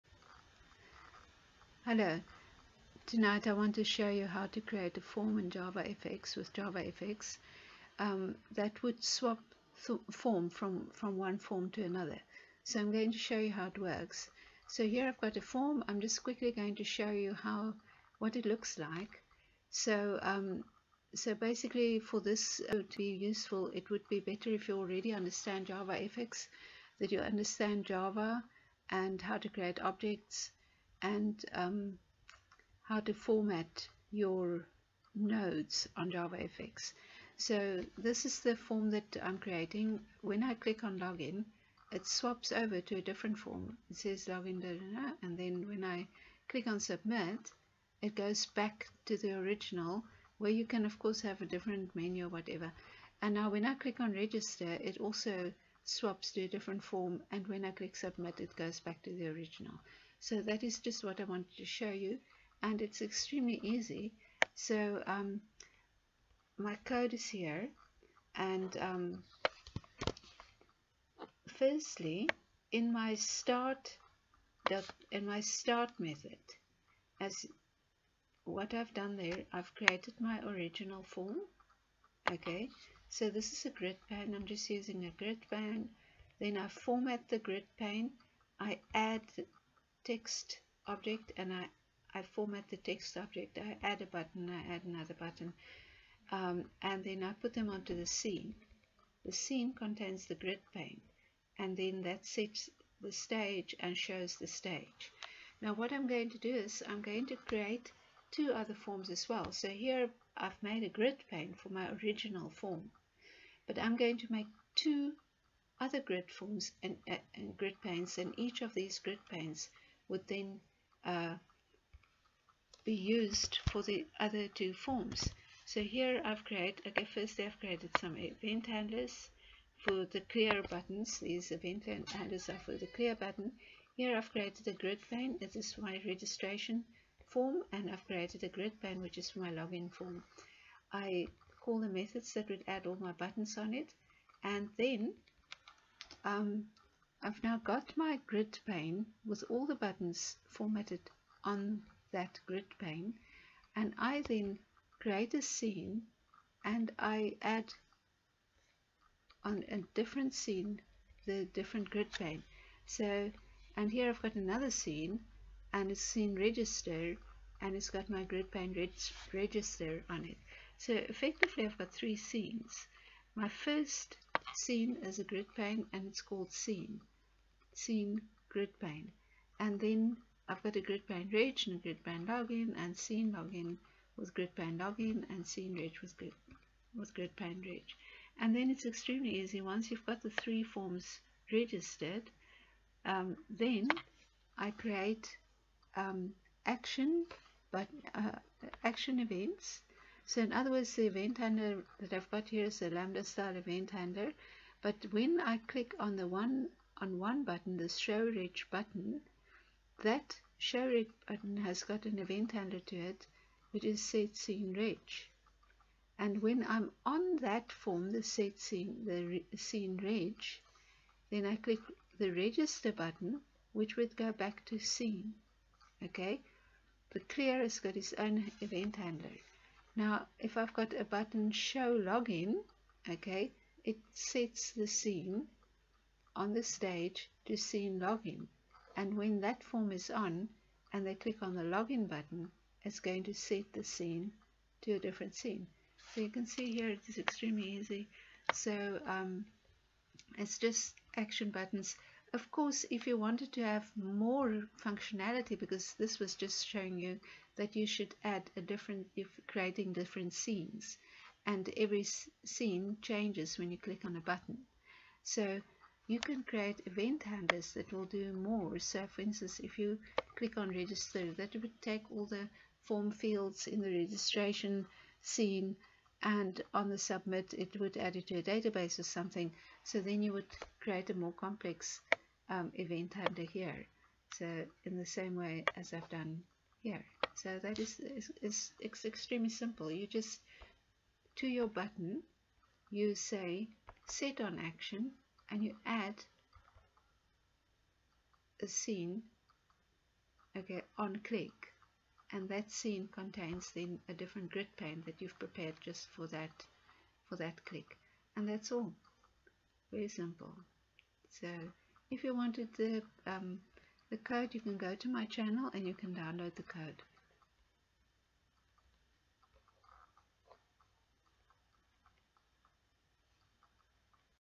Change Scene on Click using JavaFX
This video will show you how to change a scene on the event MOUSE_CLICKED. It means when your end users click a button, the form will be swapped out and a new form will appear. This is handy when you have registration procedures or subscriber areas will various functionalities for the subscribers.